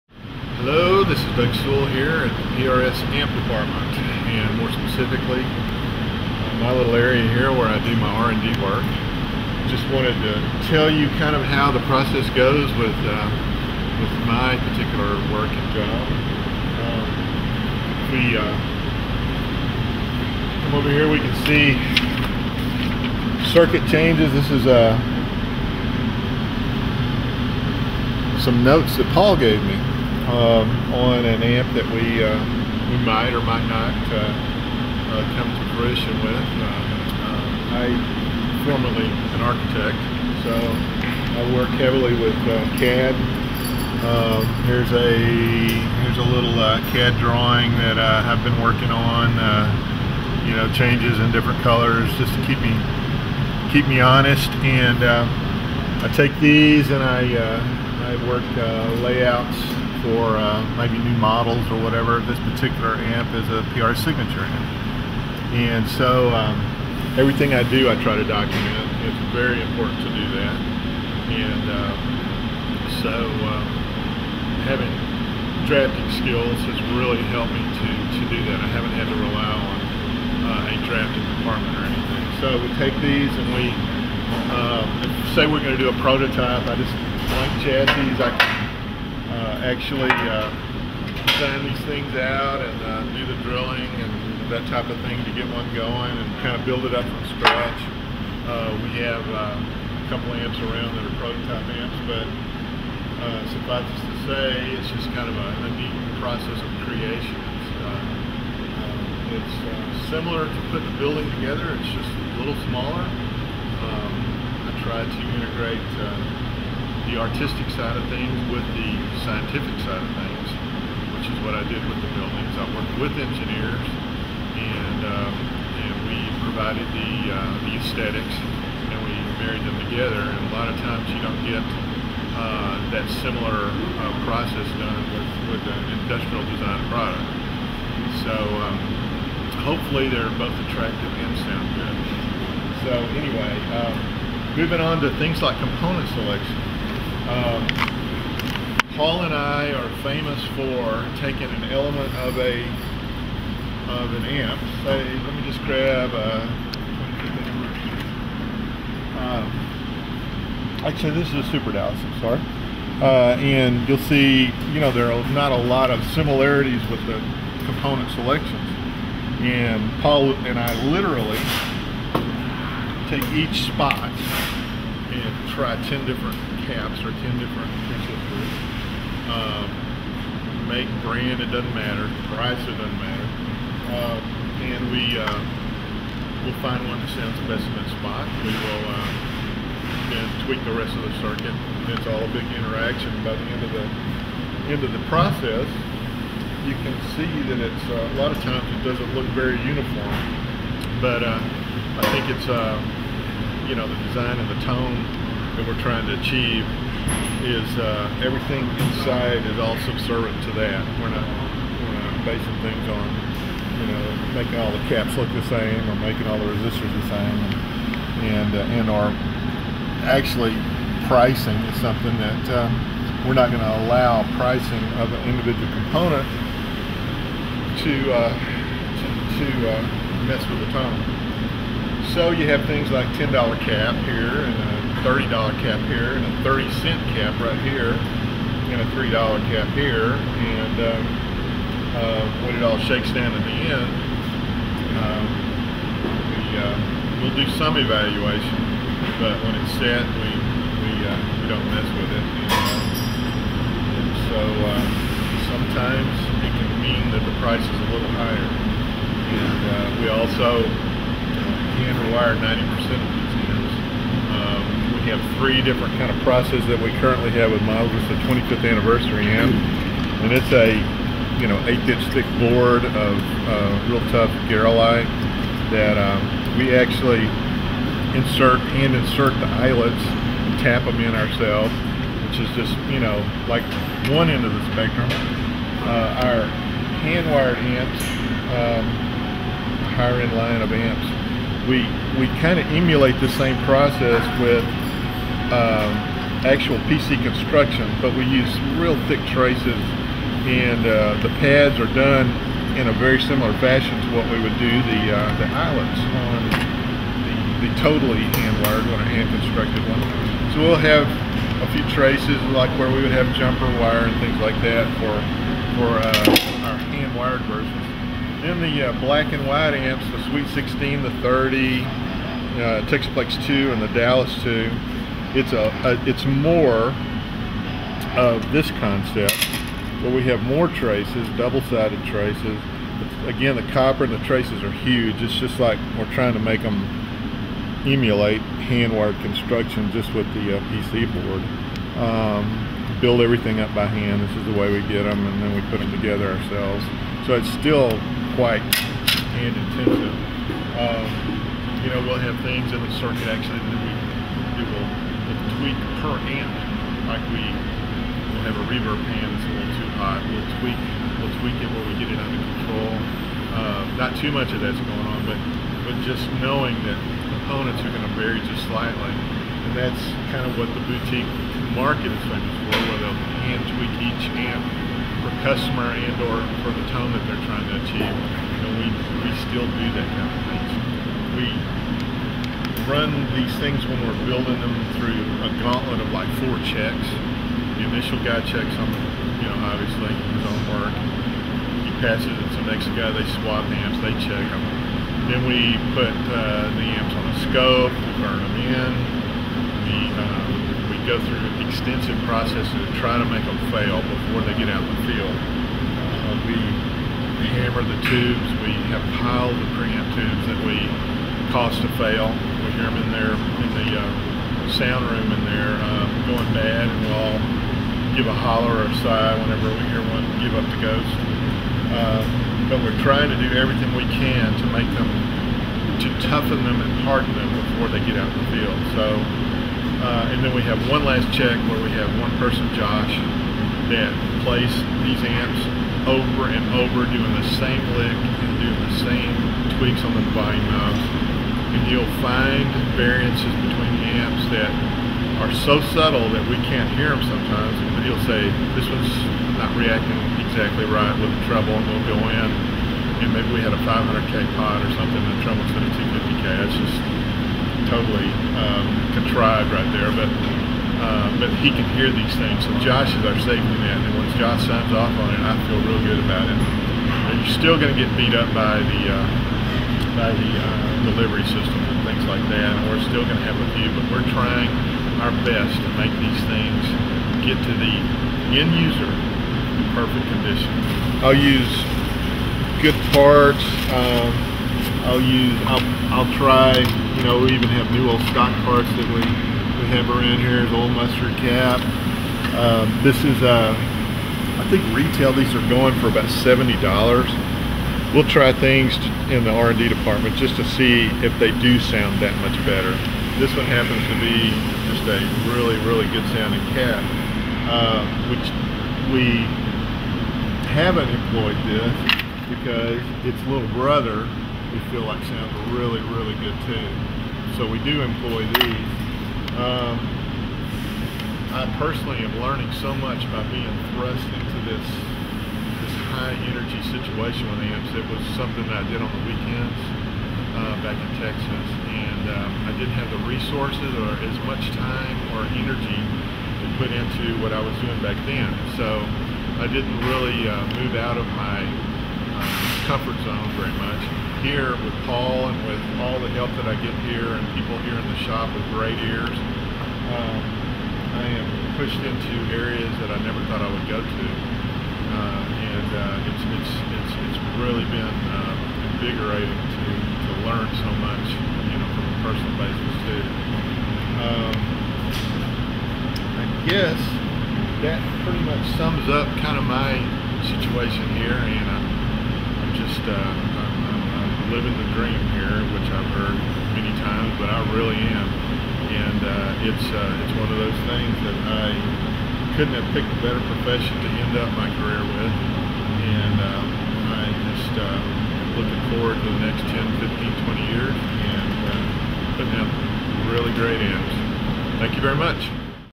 Design and Component Selection - PRS Amplifiers - Shop In Detail
PRS Amp Designer Doug Sewell explains his design process.